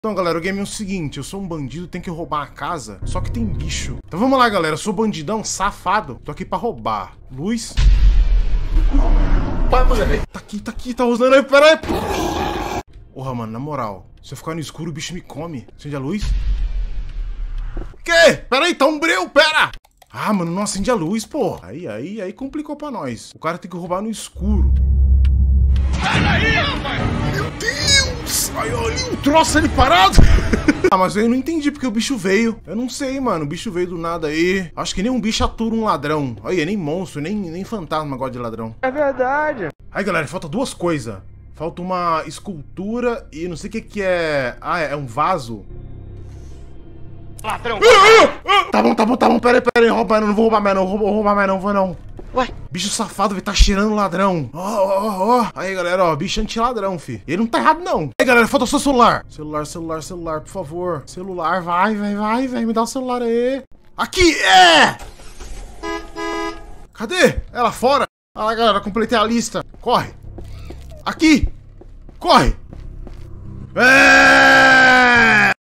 Games Eduuu e a casa dos fantasmas (animação Games Eduuu)
Animação feita a partir do vídeo do Games Eduuu onde ele joga um game chamado Boo Men.